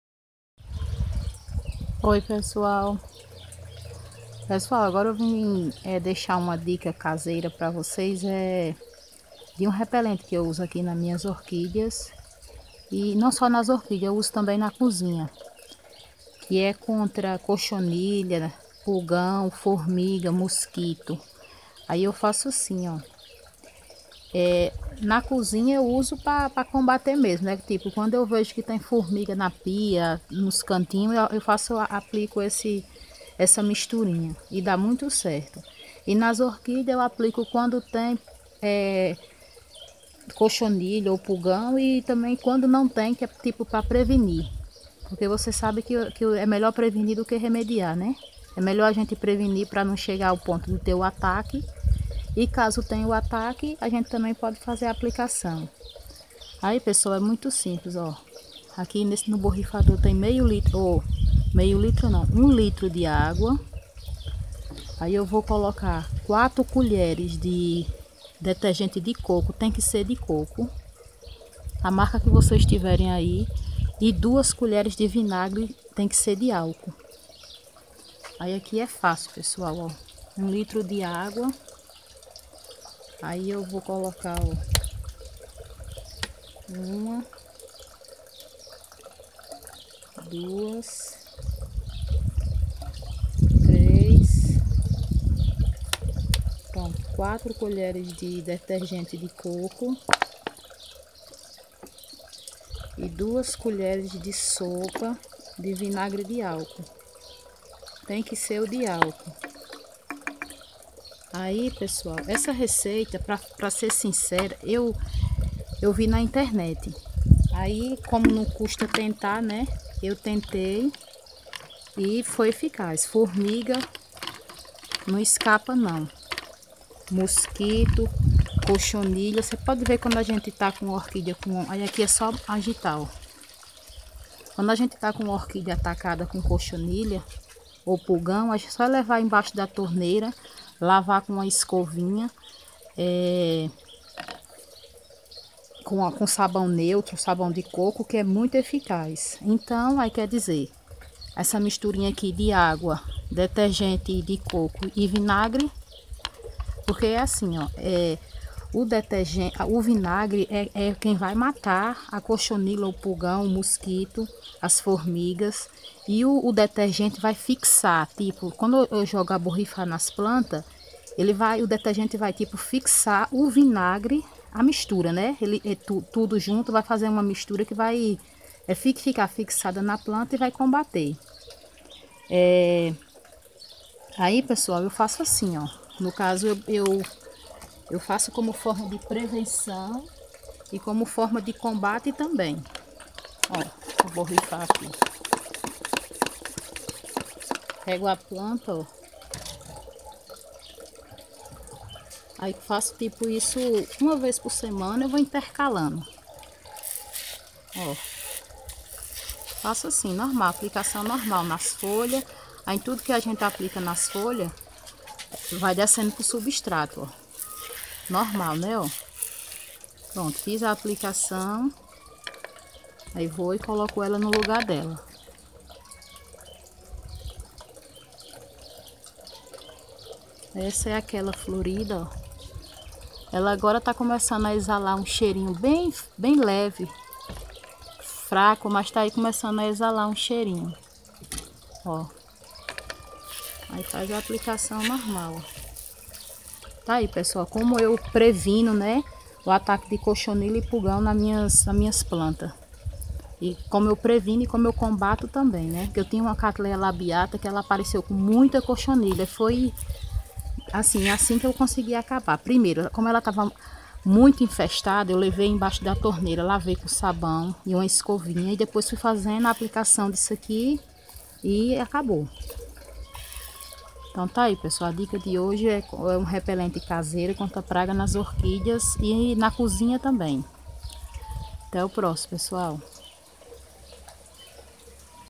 Repelente Caseiro contra Pulgão e Cochonilha nas Orquídeas.
Oi pessoal!
Espero muito que vcs gostem dessa dica caseira contra Pulgão, Cochonilha e formiga nas Orquídeas e na cozinha também...😘